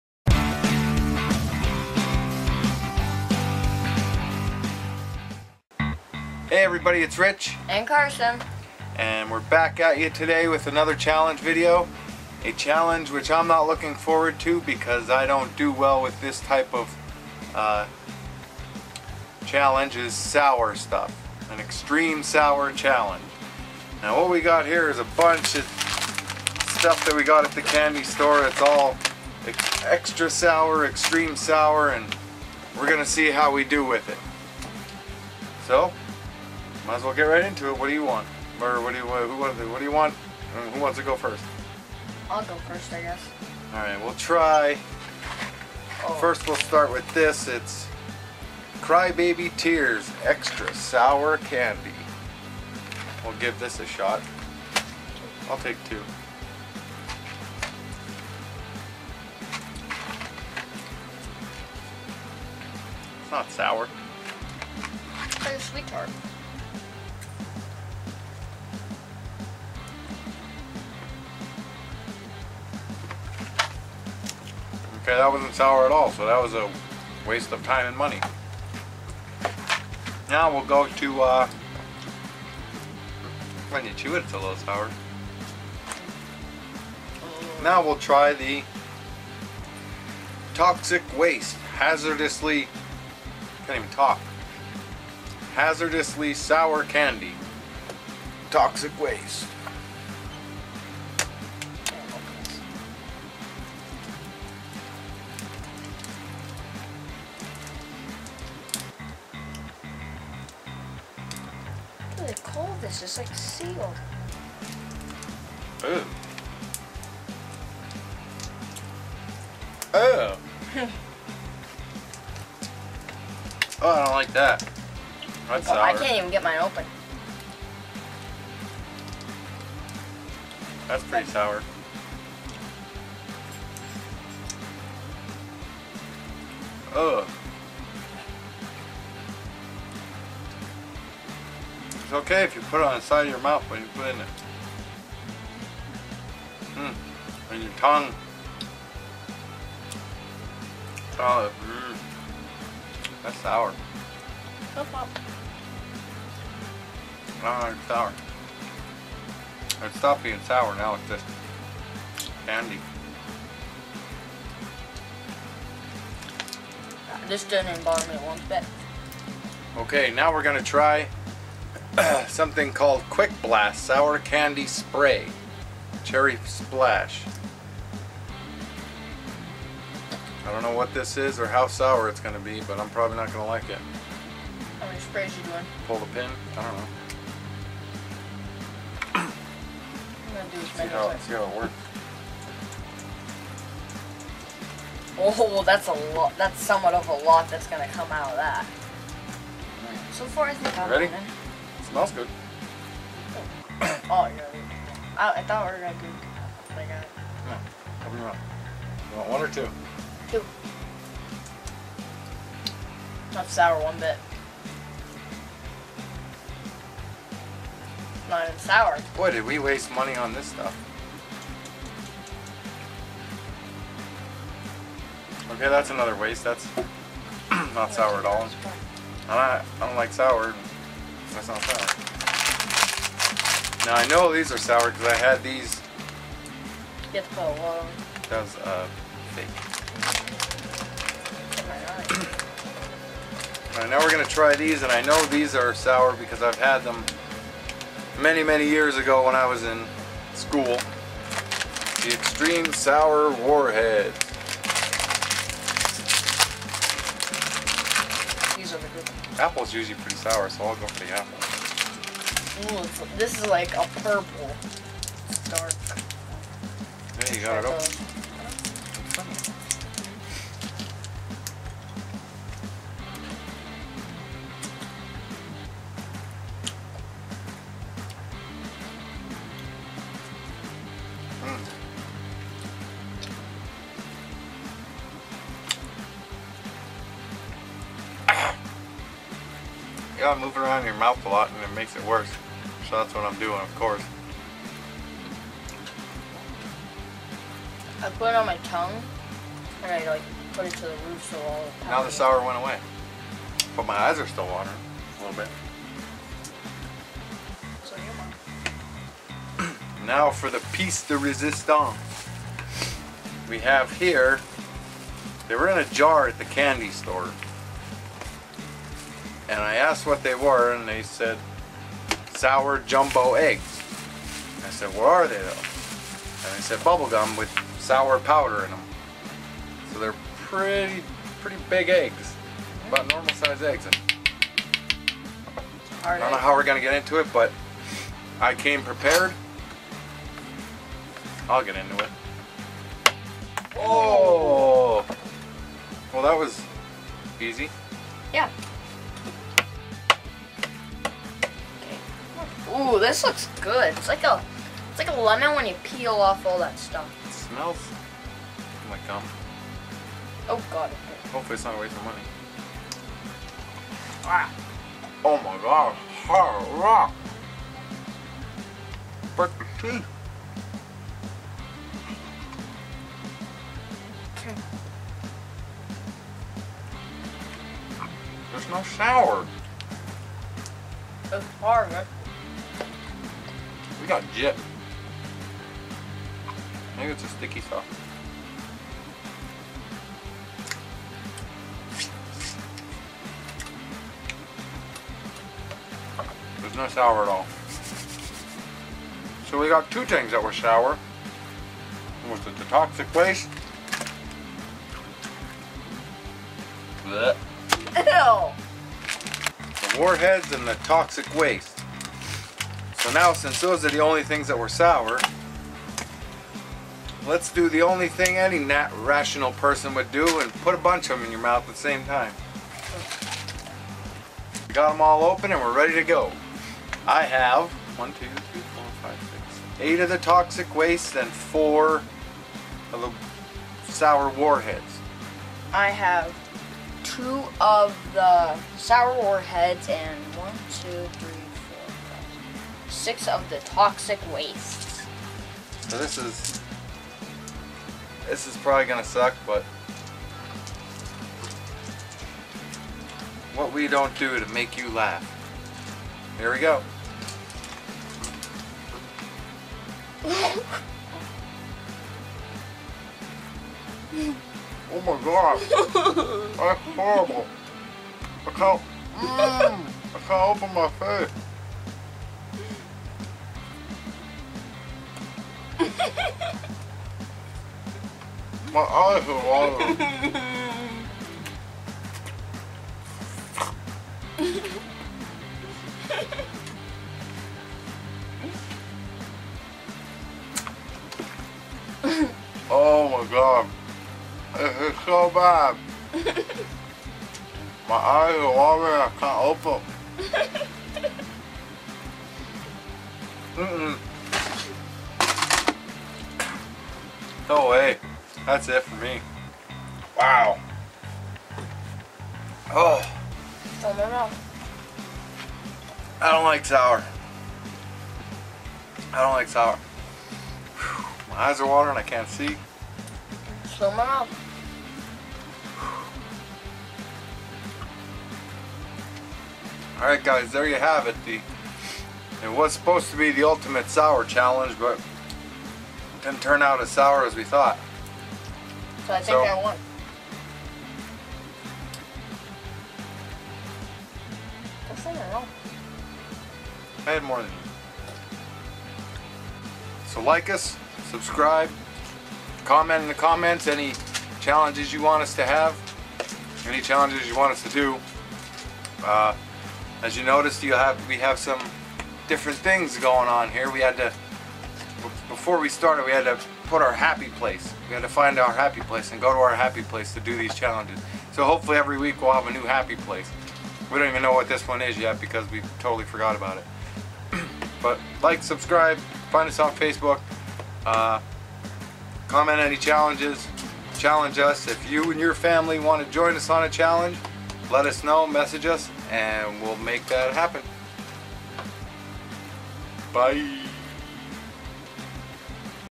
Ultimate SOUR Challenge
In this video, we take on the ULTIMATE SOUR Challenge...or so we think. Yes, I know, the whole "green/pink screen" effect hasn't worked out yet, but it will be fixed by the next video!!